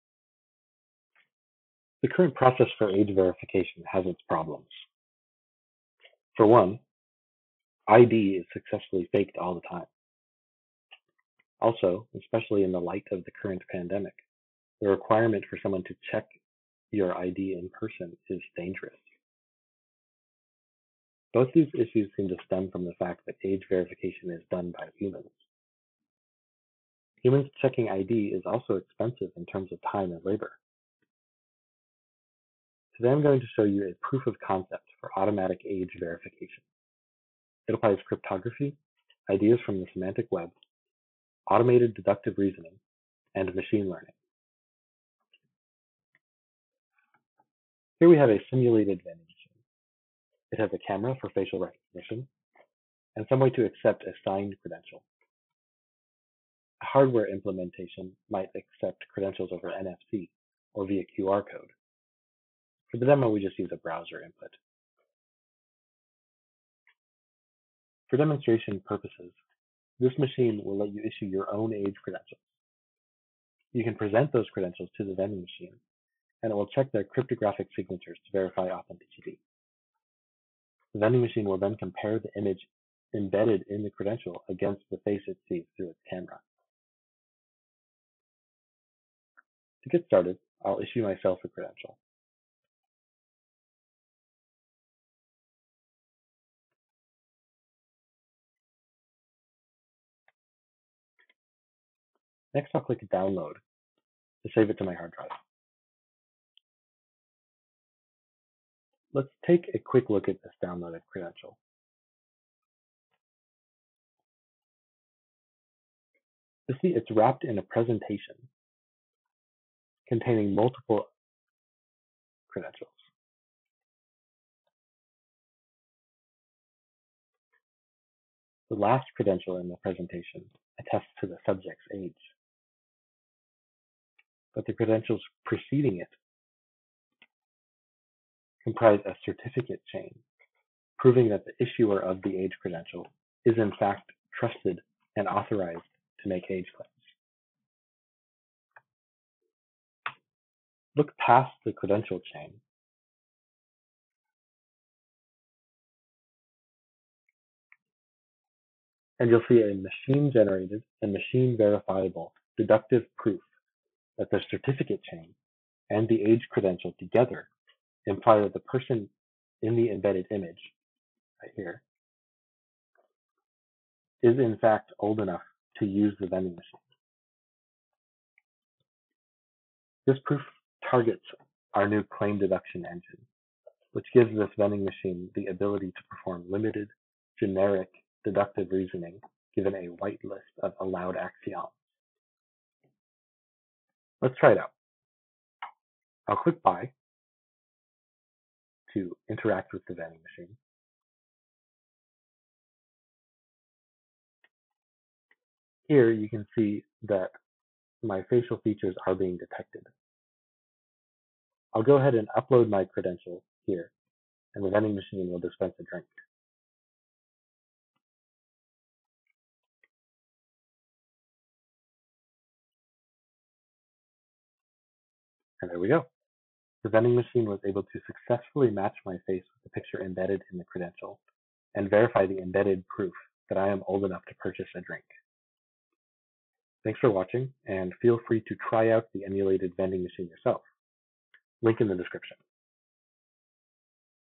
Dock claim deduction demo
Andrew Dirksen, one of Dock’s core developers, walks you through a demo that shows how claim deduction, facial recognition, and verifiable credentials can be used together to cryptographically verify a person's age, eliminating the need for manual checks.